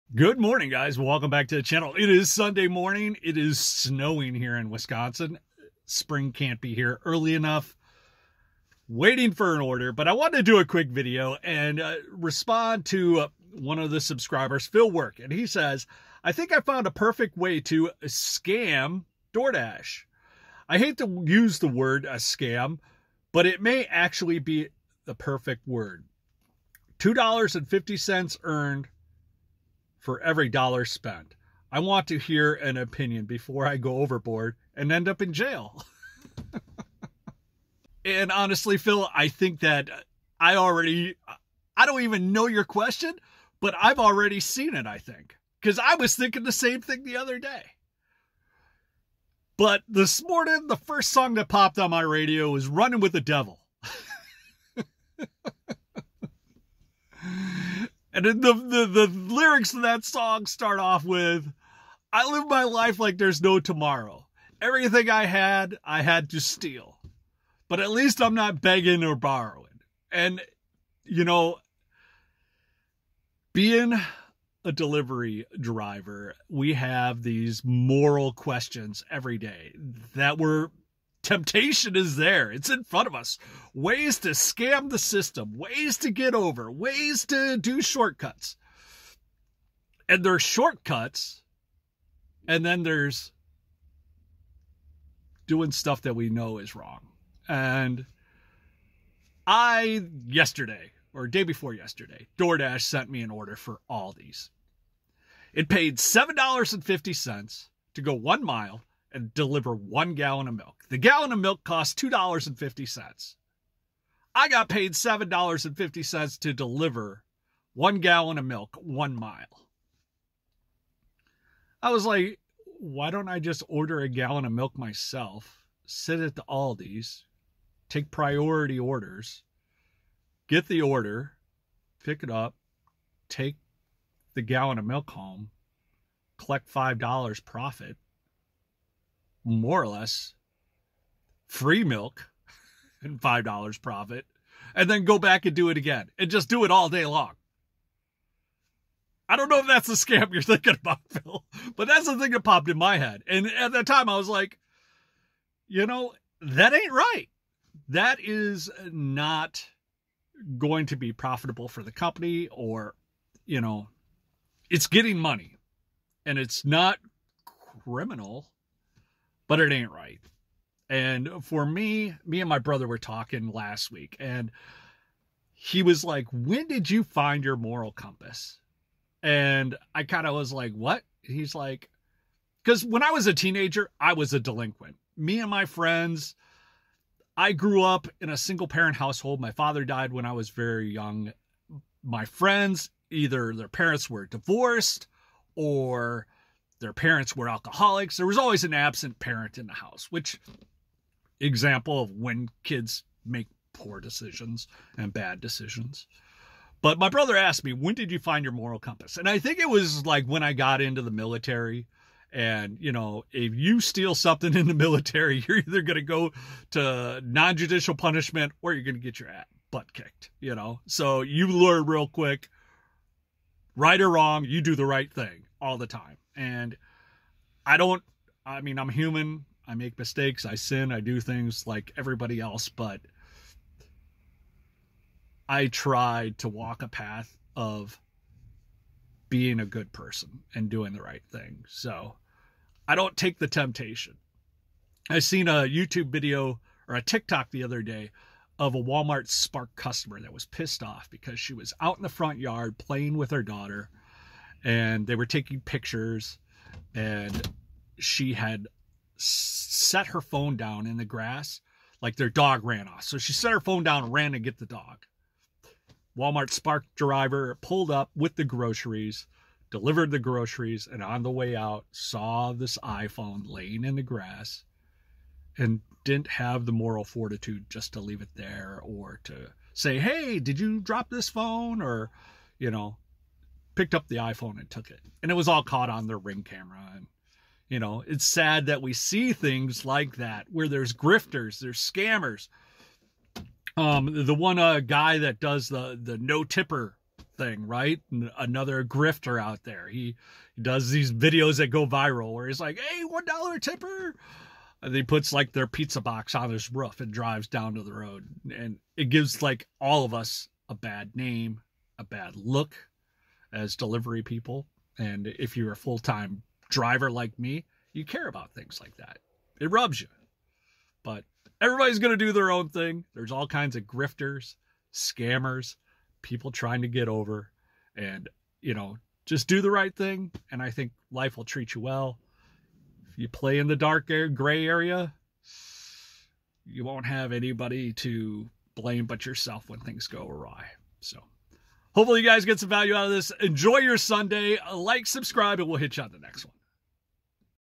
Grifters, Scammers and Thieves with Doordash and Spark.
The Midwest express shares's thoughts opinions on Grifters, Scammers and Thieves in the gig industry.